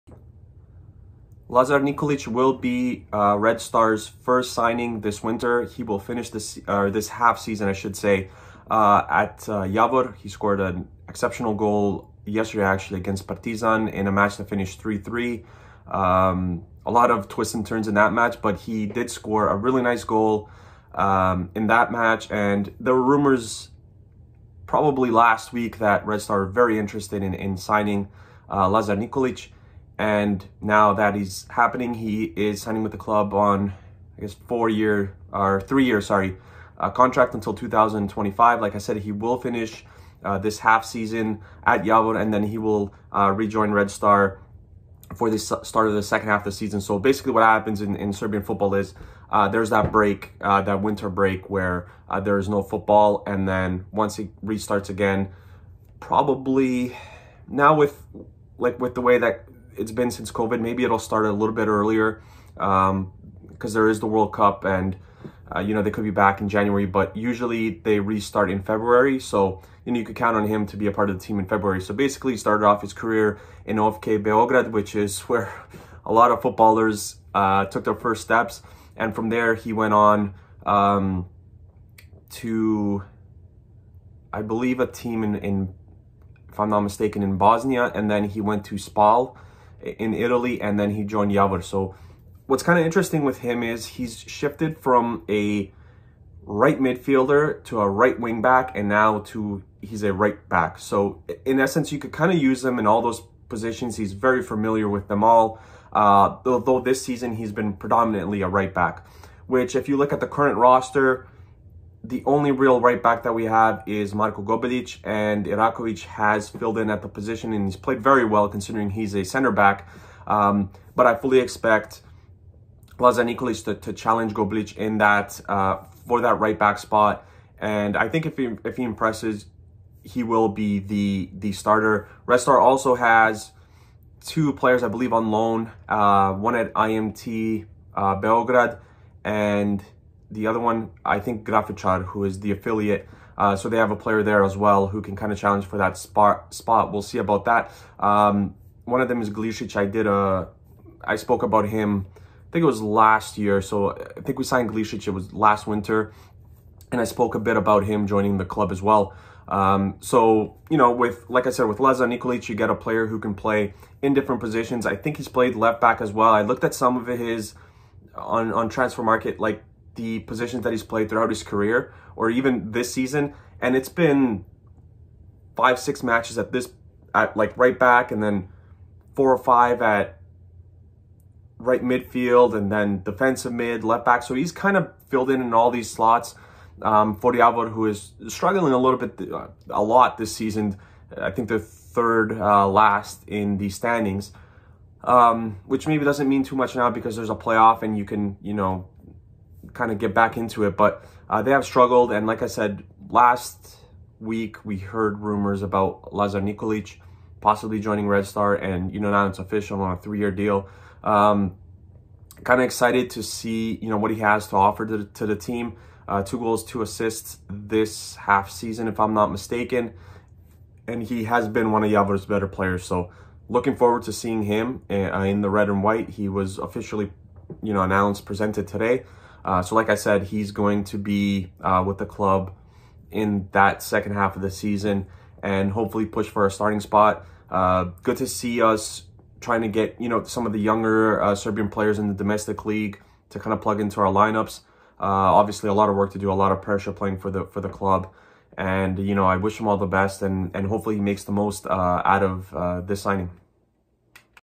LAZAR NIKOLIĆ JOINS RED STAR BELGRADE!
Red Star has signed Lazar Nikolić from FK Javor on a three year deal

Opinion based podcast and vlog about the latest happenings in the sports world, most notably soccer and basketball